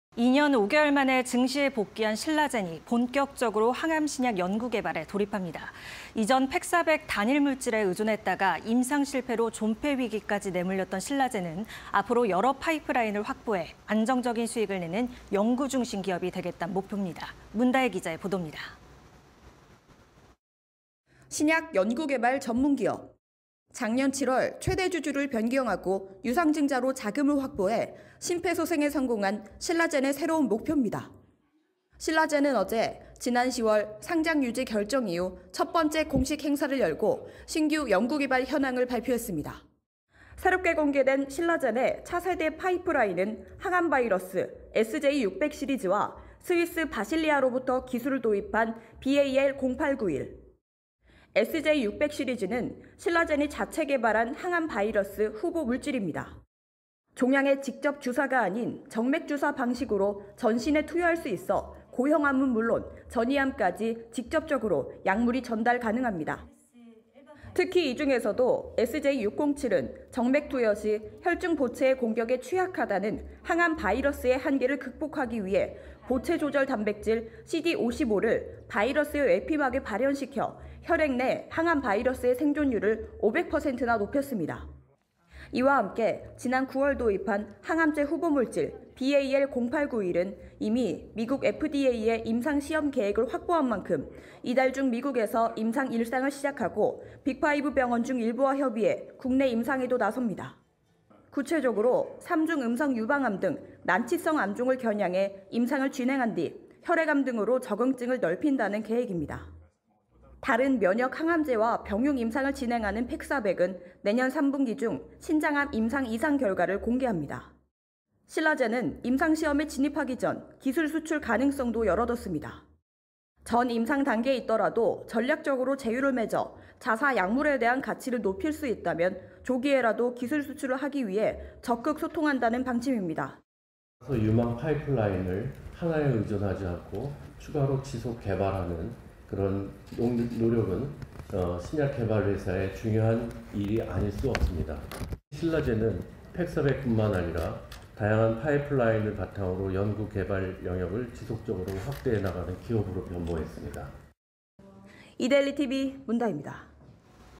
거래재개 신라젠 암 정복 재도전... 신약 개발 시동 (20221214)
거래재개 신라젠 암 정복 재도전... 신약 개발 시동